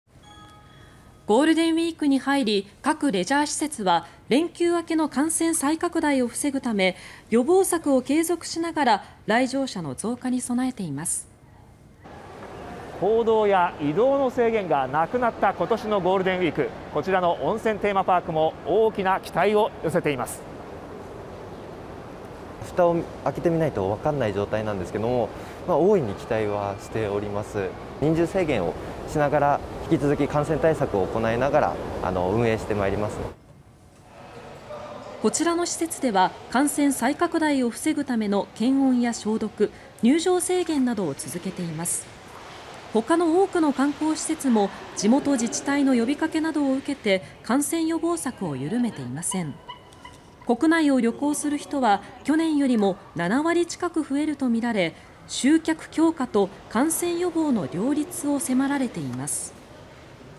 レジャー施設は感染予防策を継続 GW後の再拡大防止(2022年4月29日)
ゴールデンウィークに入り、各レジャー施設は連休明けの感染再拡大を防ぐため、予防策を継続しながら来場者の増加に備えています。

　箱根小涌園ユネッサン営業課（広報担当）・神野天洋さん：「ふたを開けてみないと分からない状態ですが、大いに期待はしております。人数制限をして、引き続き感染対策を行いながら運営して参ります」

　こちらの施設では、感染再拡大を防ぐための検温や消毒、入場制限などを続けています。

　他の多くの観光施設も地元自治体の呼び掛けなどを受けて、感染予防策を緩めていません。

　国内を旅行する人は去年よりも7割近く増えるとみられ、集客強化と感染予防の両立を迫られています。